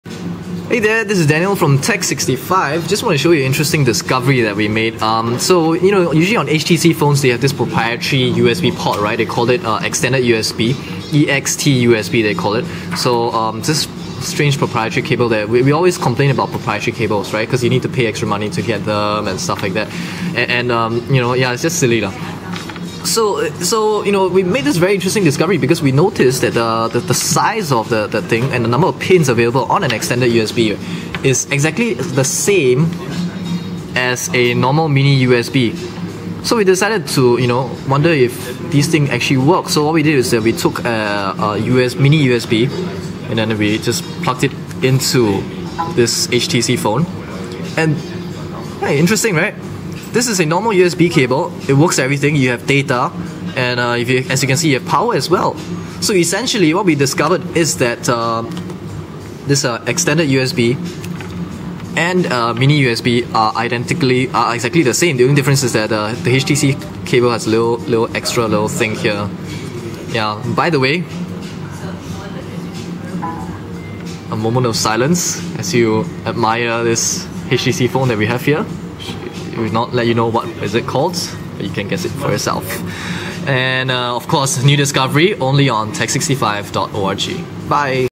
HTC's ExtUSB port is just a mini-USB port?
DK and I made an interesting discover with this HTC phone we were playing. Out of curiosity, we tried to plug in an ordinary mini USB cable into the port (don't worry HTC, we're professionals, we know how to measure ports and not force something into a whole if it's not meant to...), and without any difficulty at all, as if it was designed to, the mini USB cable plugged right into the port, providing both power and data!

Hmmm.